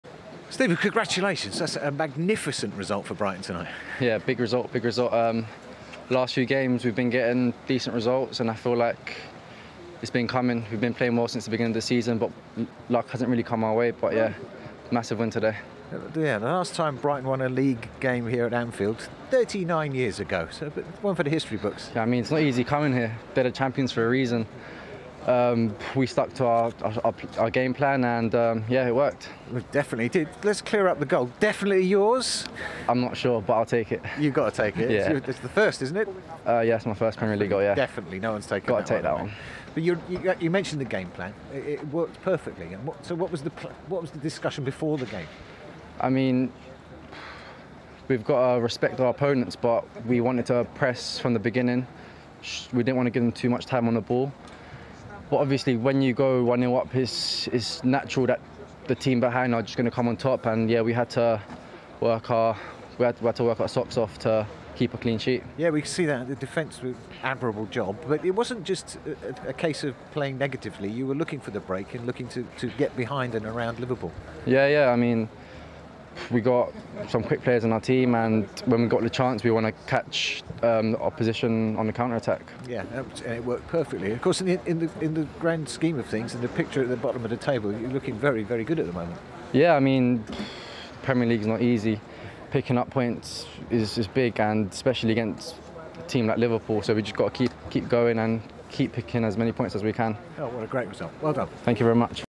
"I'm not sure but I'll take it!" Alzate happy to claim Brighton's winning goal at Anfield!
Brighton midfielder Steven Alzate spoke to Des Kelly after his side's historic Premier League win over Liverpool at Anfield.